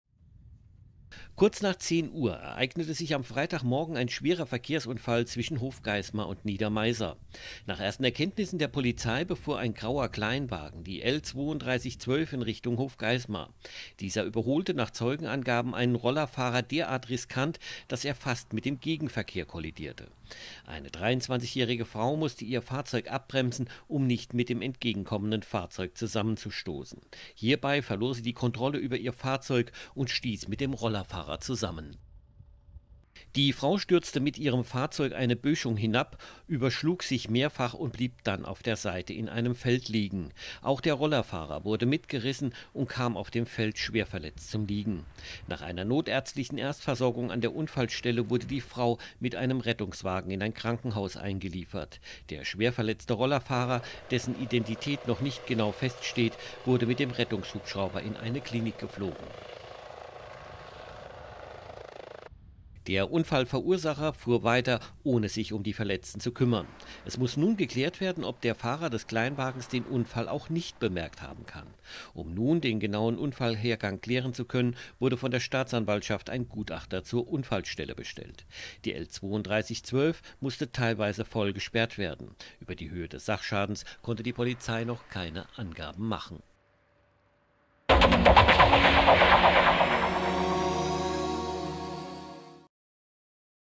Unfall nahe Hofgeismar: Rollerfahrer in Lebensgefahr
Hofgeismar / Niedermeiser, 28. November 2014. . Nach einem schweren Unfall auf der Landesstraße zwischen Hofgeismar und Niedermeiser schwebte ein 35 Jahre alter Mann aus Bad Arolsen am Freitag in Lebensgefahr.